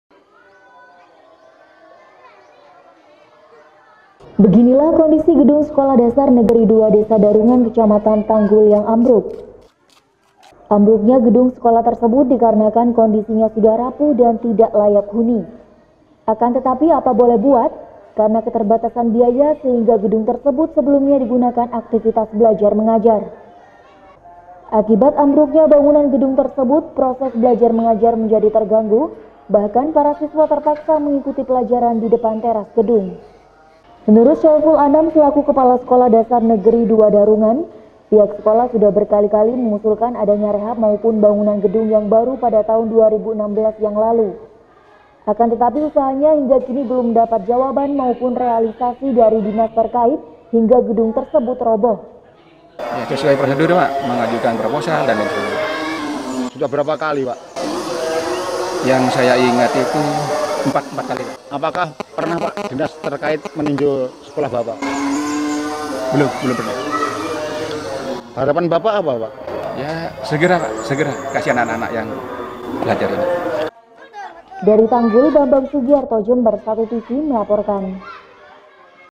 Gedung SDN 2 Darungan Ambruk
Sebuah gedung di SDN 2 Desa Darungan Kecamatan Tanggul ambruk, ambruknya gedung tersebut karena kondisinya sudah tidak layak huni, beruntung tidak ada korban jiwa saat kejadian, namun kerugian ditafsir mencapai puluhan juta rupiah.